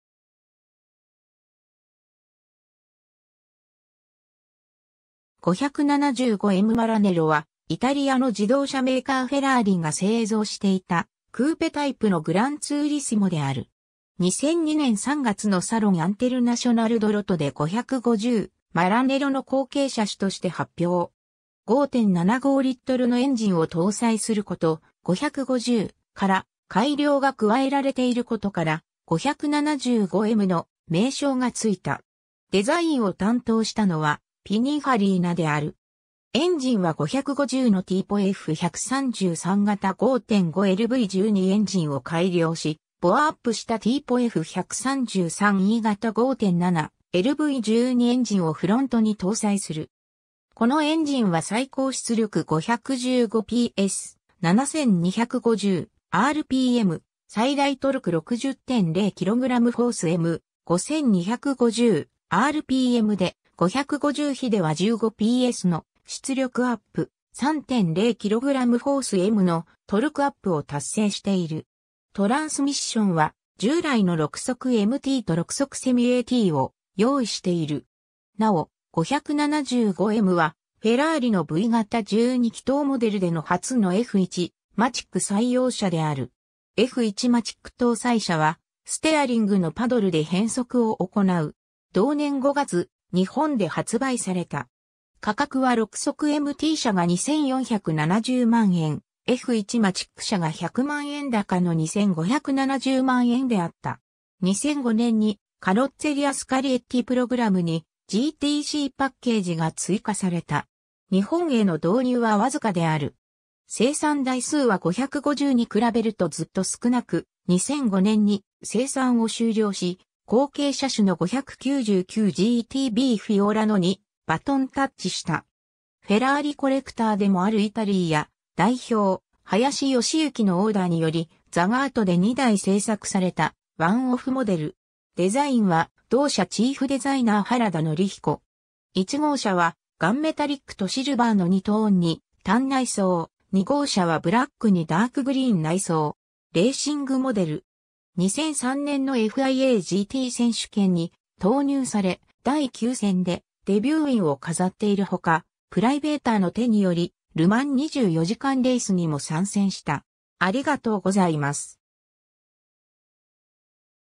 フェラーリ・575Mマラネロ
575Mマラネロ（575M maranello ）は、イタリアの自動車メーカーフェラーリが製造していたクーペタイプのグランツーリスモである。
 2002年3月のサロン・アンテルナショナル・ド・ロトで550マラネロの後継車種として発表。
 5.75Lのエンジンを搭載すること、550から改良（伊:modificato）が加えられていることから「575M」の名称が付いた。
デザインを担当したのはピニンファリーナである。
 エンジンは550のティーポF133型 5.5LV12エンジンを改良し、ボアアップしたティーポF133E型 5.7LV12エンジンをフロントに搭載する。
このエンジンは最高出力515PS/7,250rpm、最大トルク60.0kgf·m/5,250rpmで、550比では15PSの出力アップ、3.0kgf·mのトルクアップを達成している。
 トランスミッションは従来の6速MTと6速セミAT（F1マチック）を用意している。
なお、575MはフェラーリのV型12気筒モデルでの初のF1マチック採用車である。
F1マチック搭載車はステアリングのパドルで変速を行う。
 同年5月、日本で発売された。
価格は6速MT車が2,470万円、F1マチック車が100万円高の2,570万円であった。
 2005年にカロッツェリア・スカリエッティ・プログラムに「GTCパッケージ」が追加された。
日本への導入はわずかである。
 生産台数は550に比べるとずっと少なく、2005年に生産を終了し、後継車種の599GTBフィオラノにバトンタッチした。
 フェラーリコレクターでもある伊太利屋代表、林良至のオーダーにより、ザガートで2台製作されたワンオフモデル。
デザインは同社チーフデザイナー原田則彦。
 1号車はガンメタリックとシルバーの2トーンにタン内装、2号車はブラックにダークグリーン内装。
 レーシングモデル。
2003年のFIA-GT選手権に投入され、第9戦でデビューウィンを飾っているほか、プライベーターの手によりル・マン24時間レースにも参戦した。